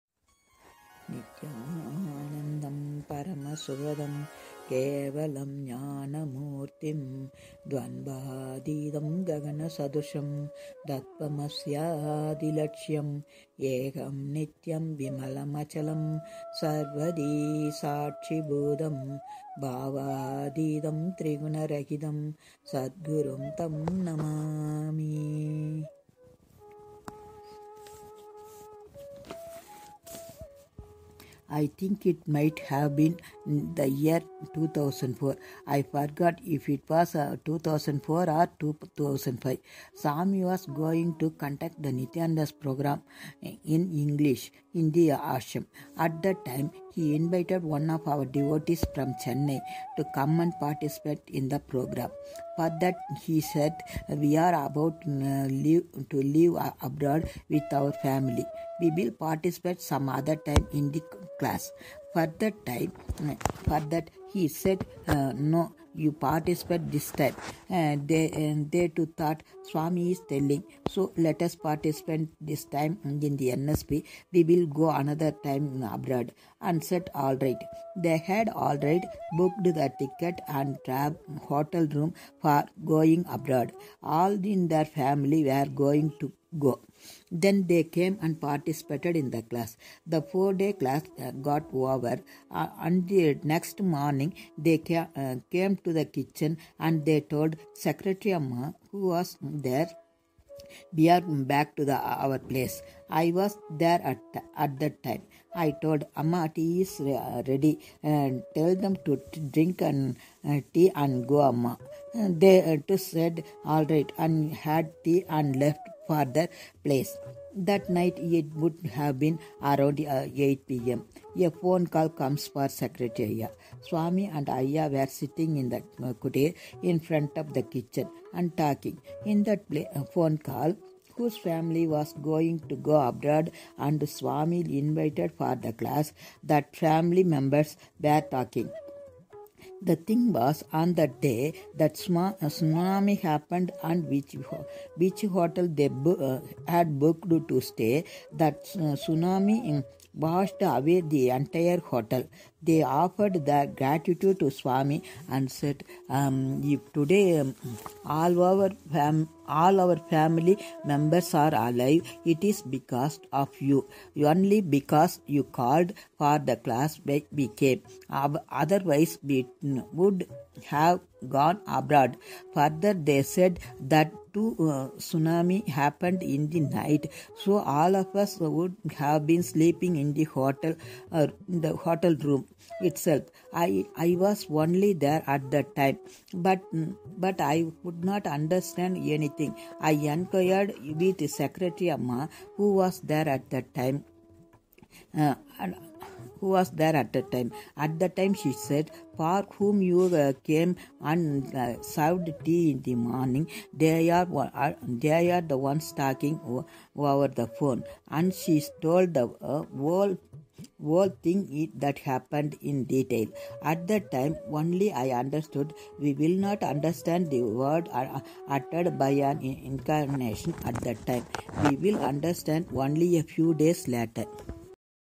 Saved from a Tsunami (English) (made with Spreaker)
Most often, the unconscious mind is veiled from the multi-dimensional reality of the Universe which The Avatar literally weilds seamlessly in His hands. Therefore,  His Divine instructions and guidance can only be truly understood in hindsight, when the purpose with which He used those choice words will reveal itself to be the most profound, often unfathomable truth.

In this audio memoir, SwamiAmma, the biological mother of the Supreme Pontiff of Hinduism, recalls how an entire family was saved from absolute devestation when they followed their Guru's words.